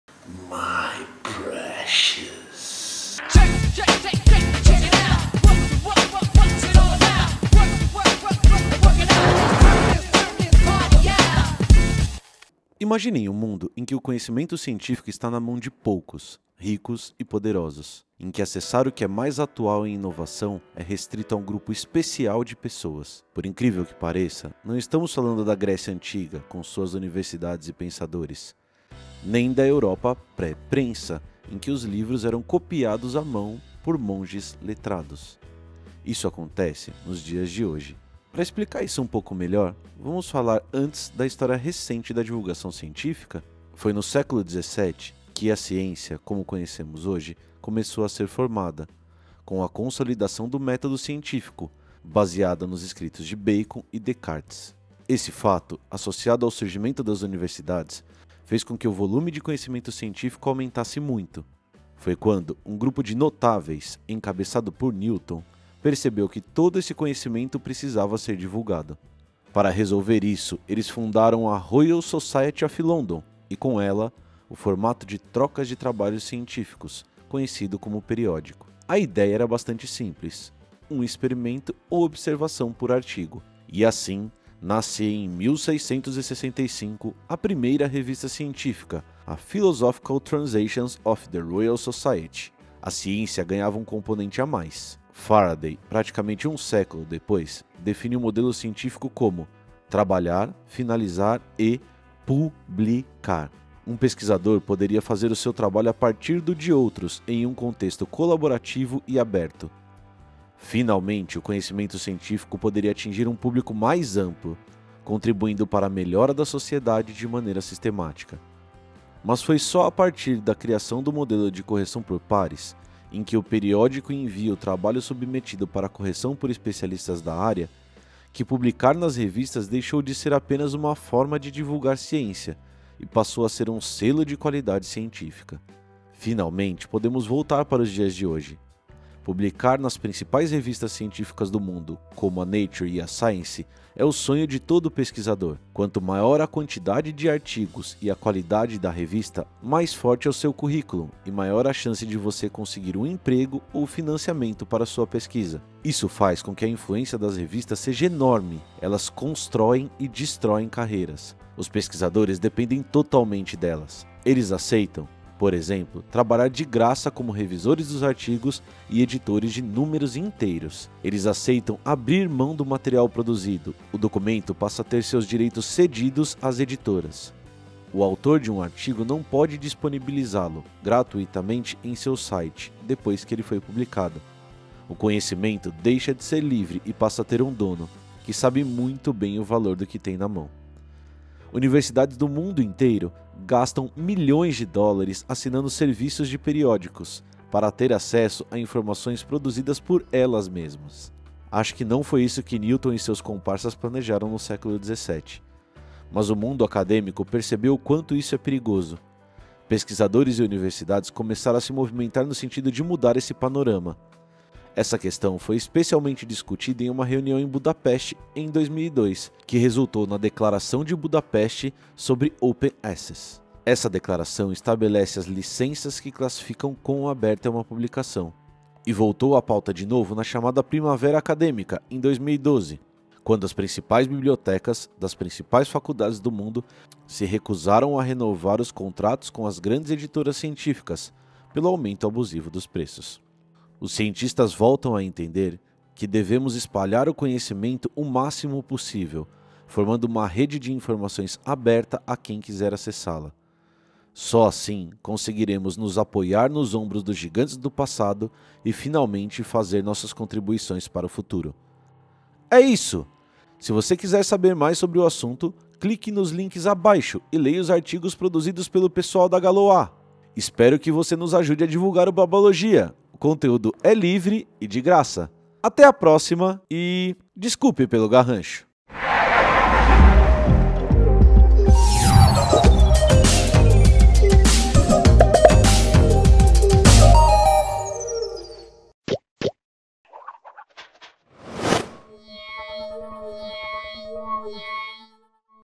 Quem são os donos do conhecimento científico? Garrancho 13
Entenda o papel das Revistas Científicas na divulgação do conhecimento hoje em dia!

Nos siga no Twitter: @BlaBlalogia

Emilio no Twitter: @piscolisco

Arte, introdução e finalização criados por Caique Oliveira// xHardbass.